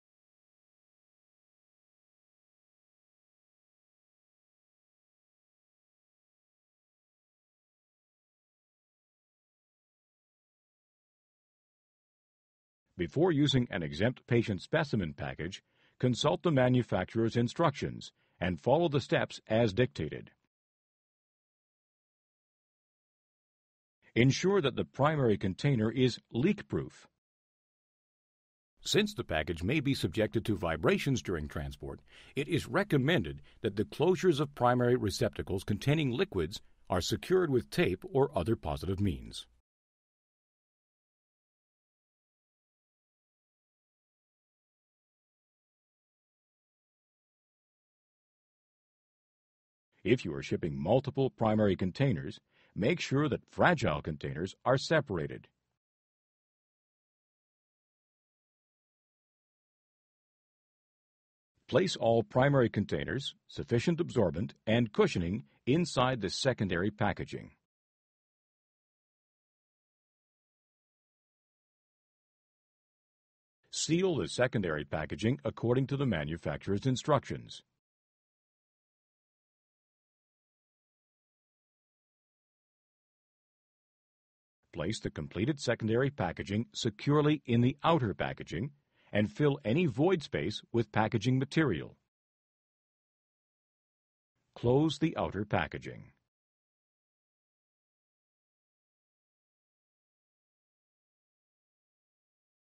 Saf-T-Pak Exempt Shipping Package
This Saf-T-Pak video outlines the basic transportation requirements and packaging in order to pack an Exempt patient specimen according to the IATA regulations section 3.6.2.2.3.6.  The packaging system used in the video is Saf-T-Pak's dual purpose STP-210. Please use proper personal protective equipment when handling infectious substances.